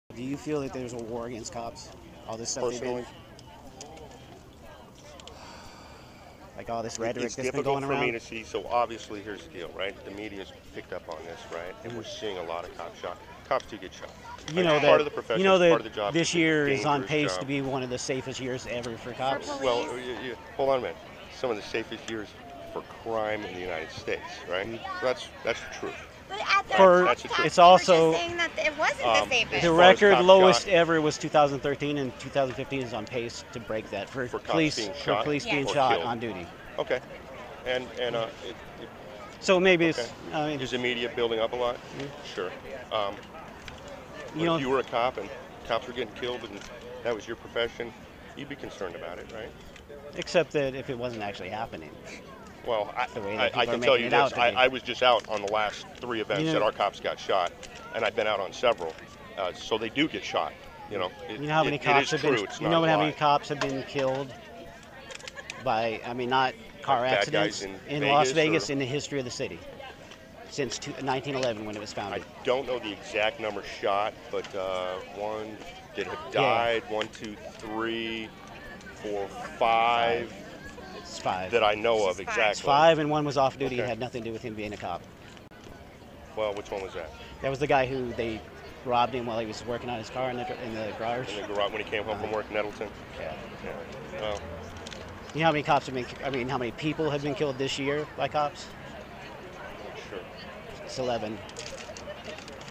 Is There Really a War Against Cops? - Dialogue at LVMPD Pep Rally 2015
Spoiler: No.
Just how many cops have been murdered in the entire history of Las Vegas? Meanwhile, how many people have been killed by cops in Las Vegas just this year? The answers are 5 and 11, respectively (and one of the cops killed was off duty and his murder had nothing to do with him being a cop). The "War On Cops" is less real than the "War On Christmas." The number of cops who have been killed (excluding car accidents, when they usually killed themselves by driving too fast and not wearing a seat belt) are at record lows the past few years. At the same time, they are killing citizens at record numbers, which are increasing every year.